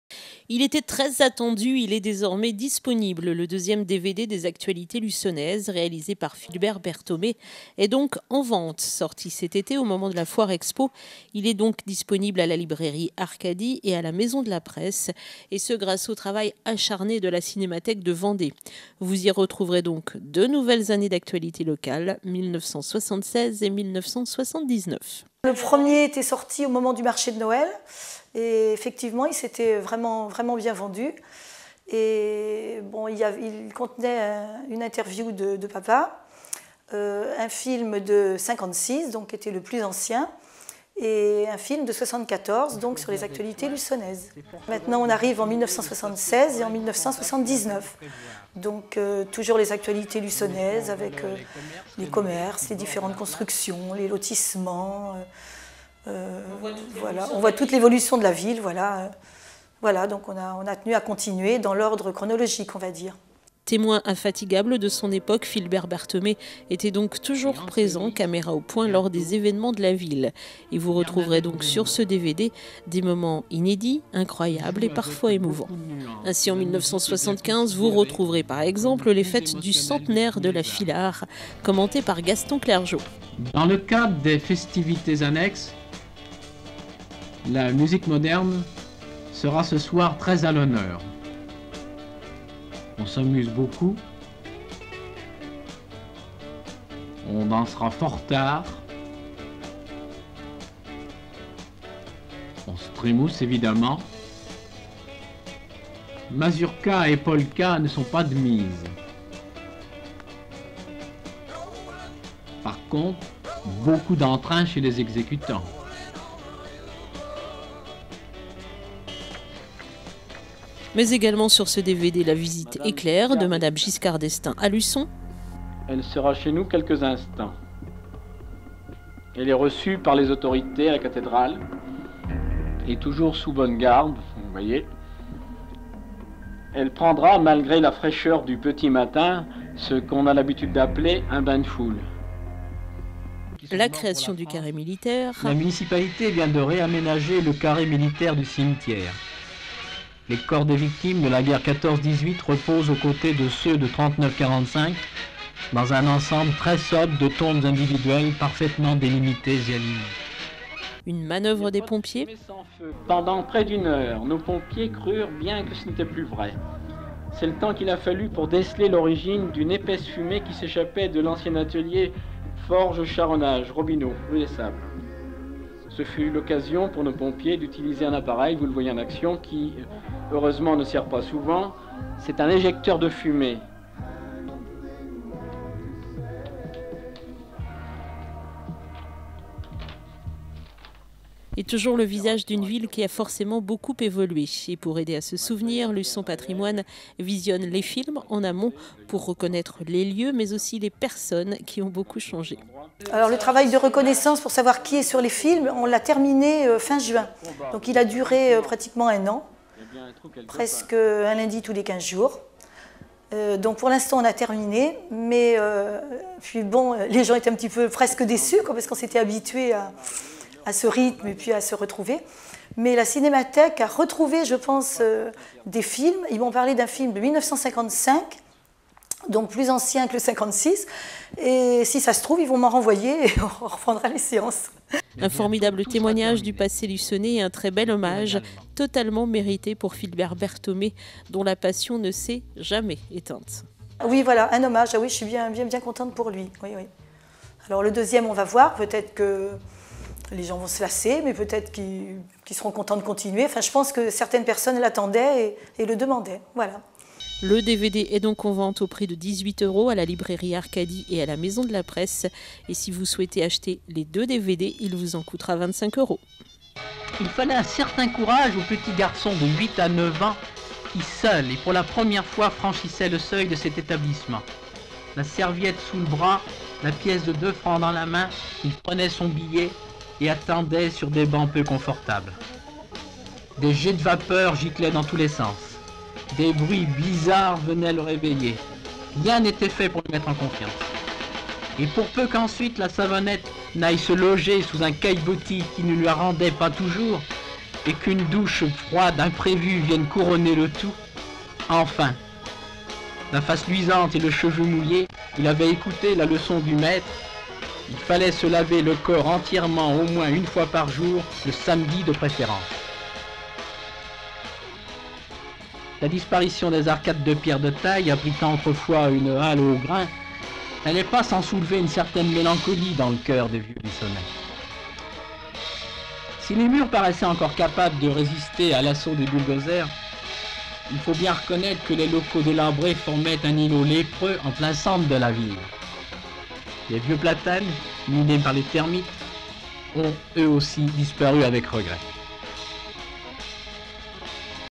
Luçon : un nouveau DVD des Actualités Luçonnaises
Le second DVD des actualités luçonnaises de Philbert Berthomé est disponible. Il regroupe les évènements de 1976 et 1979. Vous y retrouverez notamment les 100 ans de la Philhar, la construction de nouveaux lotissements, la destruction des bains-douches et des manœuvres de pompiers !